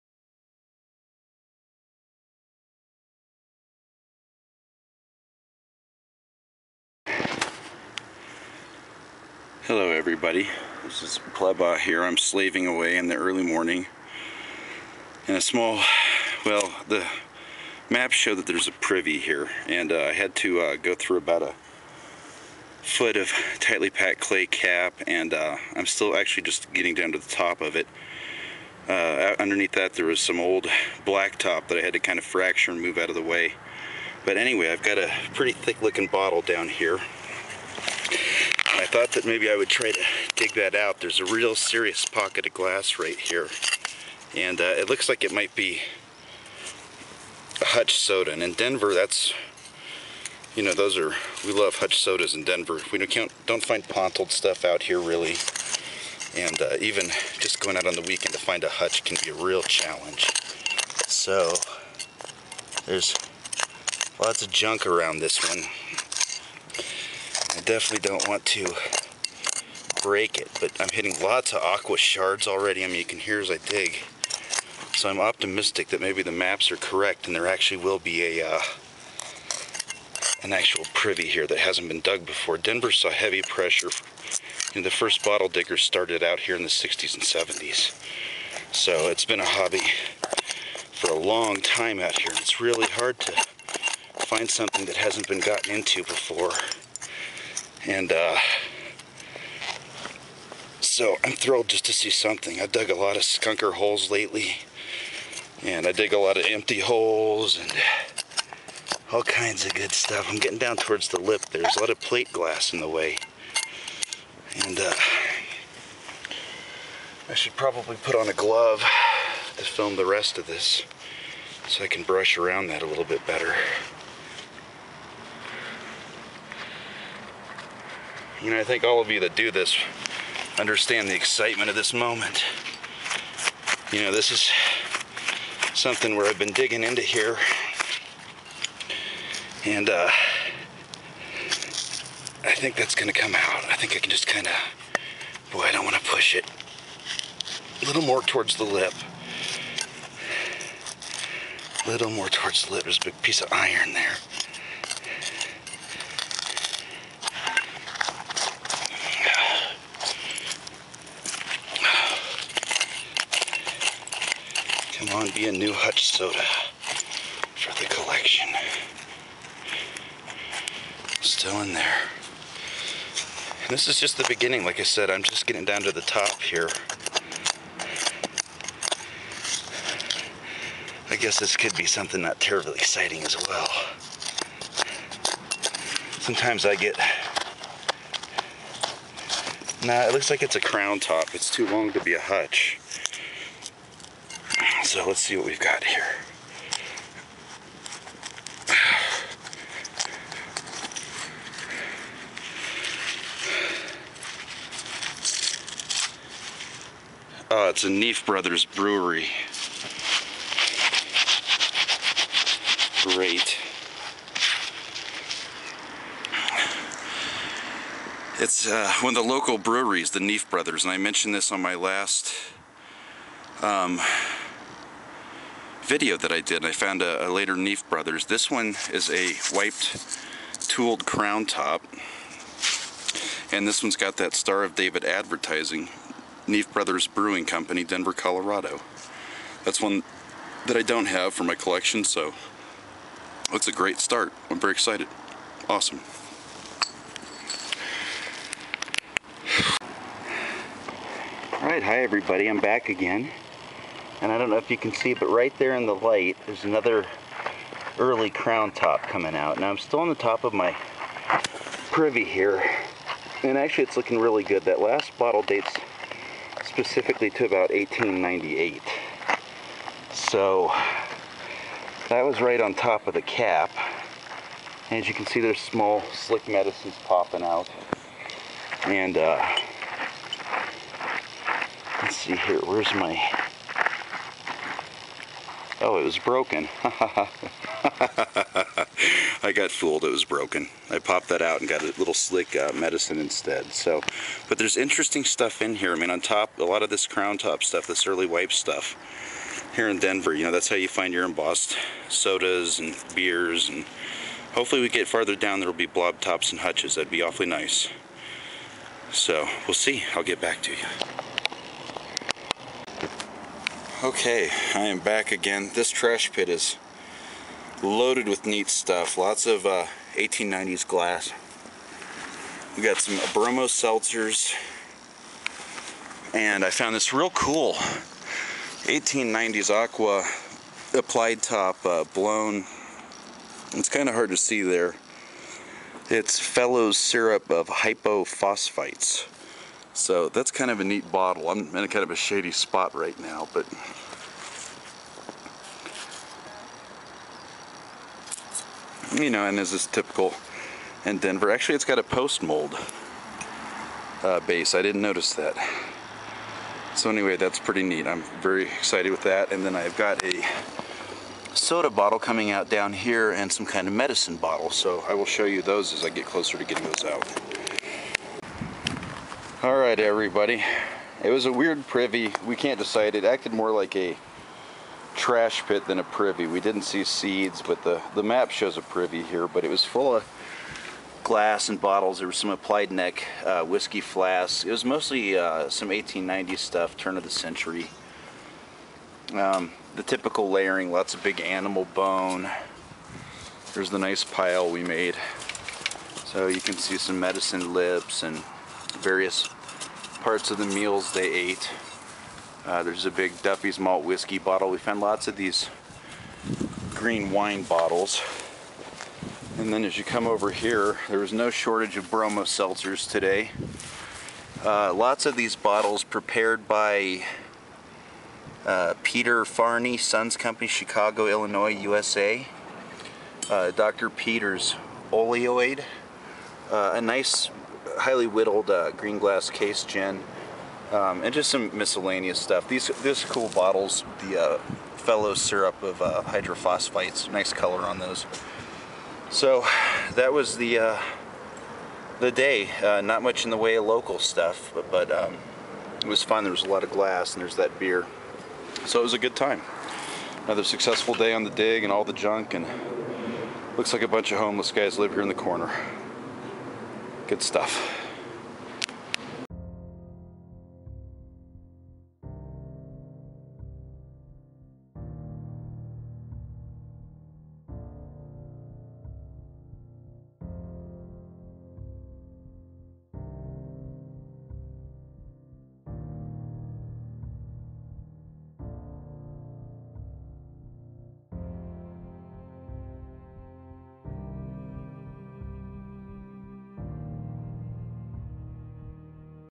Through the asphalt to the bottles
This was one of those beautiful digs of faith. The Sanborn map said it was there, but my earlier probing had yielded ugly results. It turns out that as soon as I took off about a foot of clay and cobbles, and then removed some old asphalt blacktop, that ash, animal bones, and aqua shards started to appear. It was then historic arachaeology at its best, minus the paperwork. I will sum up the results for "the taxpayers" here: the people ate chicken, pork, and beef, along with eggs. They had no children, and at least one suffered from chronic lung problems; although we found no consumption specific bottles. They also liked wine and gin in particular.